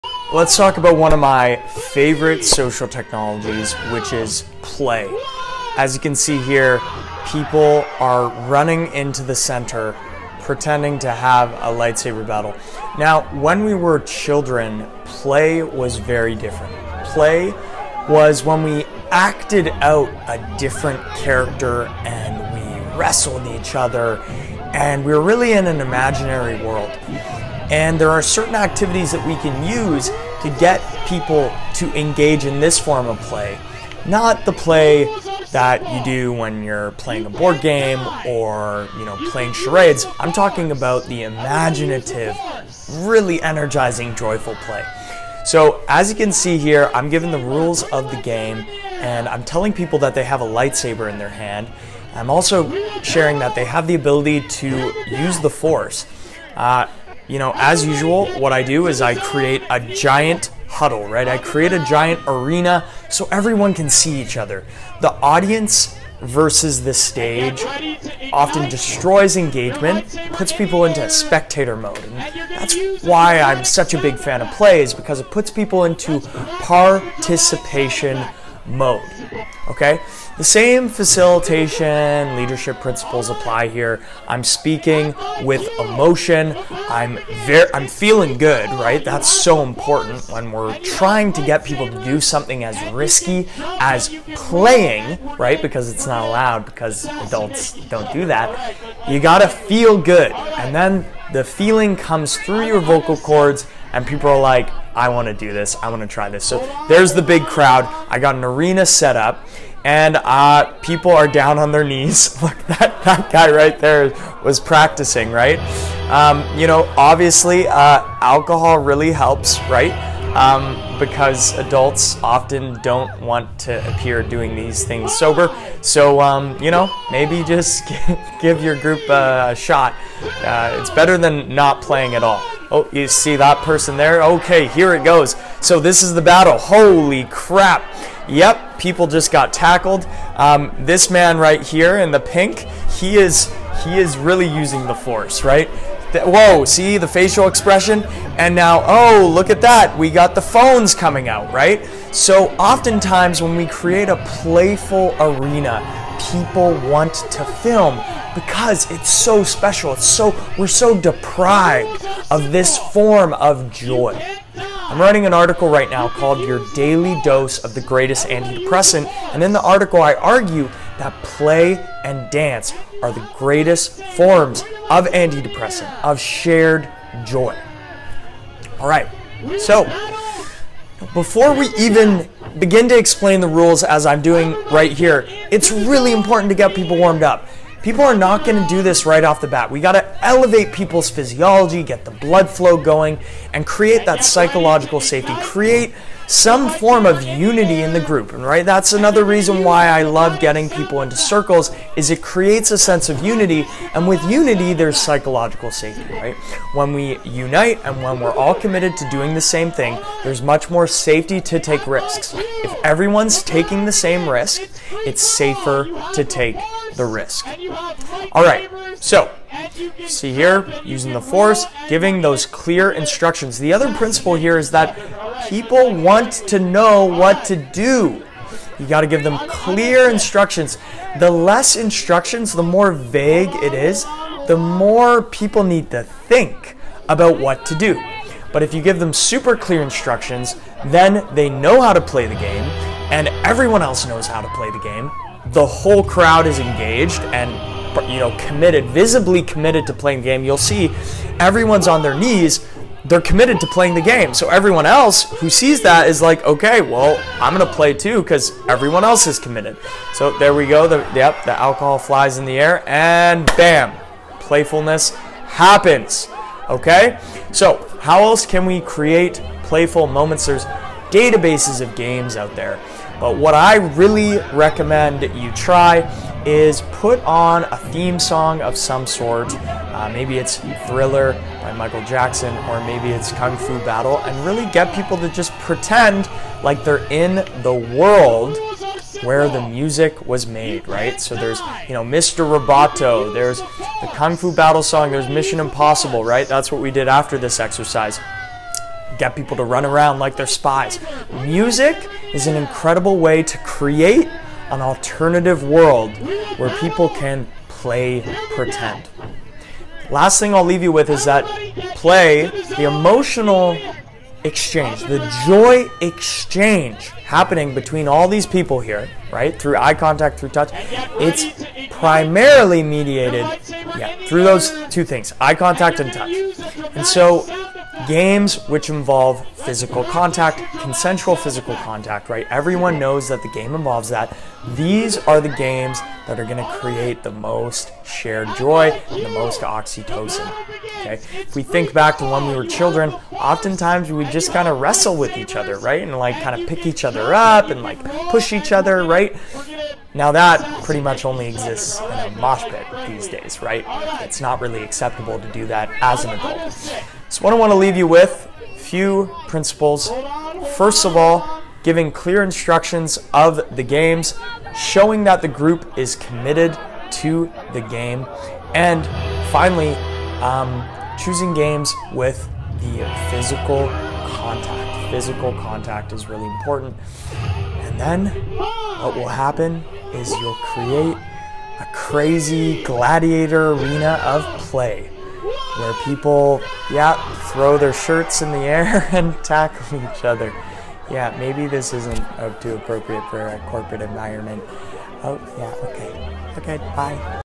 how to get randoms on a beach playing star wars together
Play is one of the greatest antidepressants. This is how you can make it happen in a group of strangers. Like I did on this random beach.

Want more instruction on becoming a legendary community facilitator?

The Party Scientist's Lab: My Personal Podcast & Newsletter 👇🏽
For Community Builders & Connection Facilitators 🥼